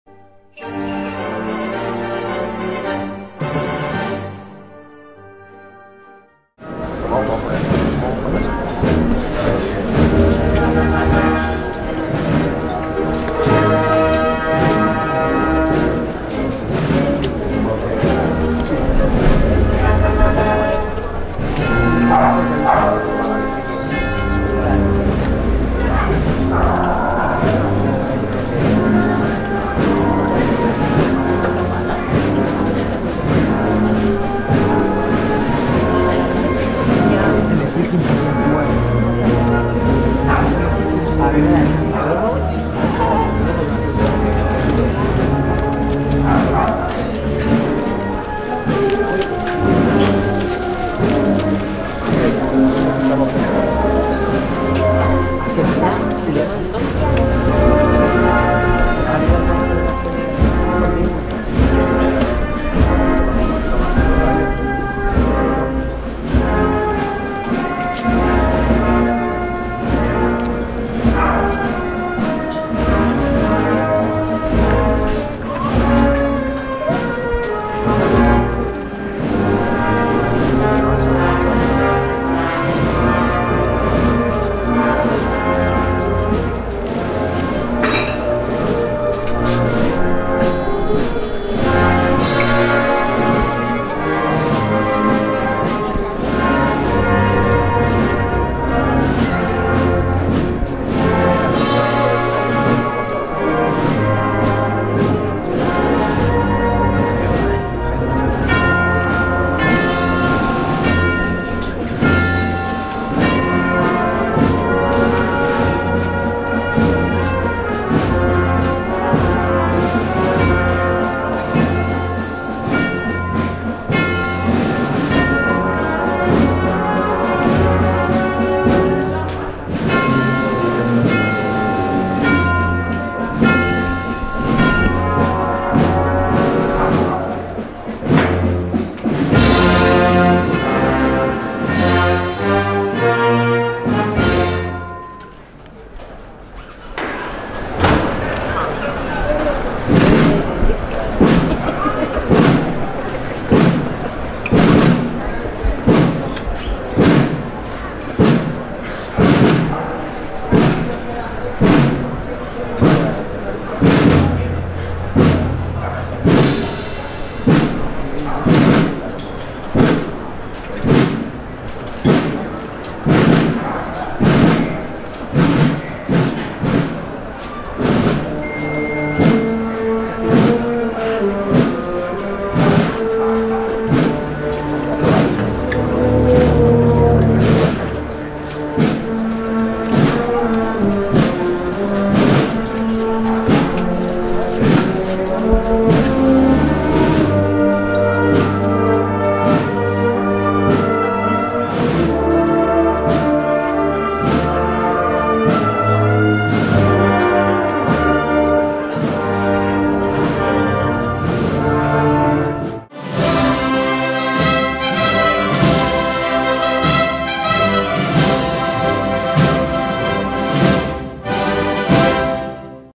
[4K] Virgen de la Amargura en Feria - Santo Entierro Grande 2023
Virgen de la Amargura de la @HdaddelaAmargura en la calle Feria, en el Santo Entierro Grande del 2023.

🥁 Acompañado por @ElCarmenDeSalteras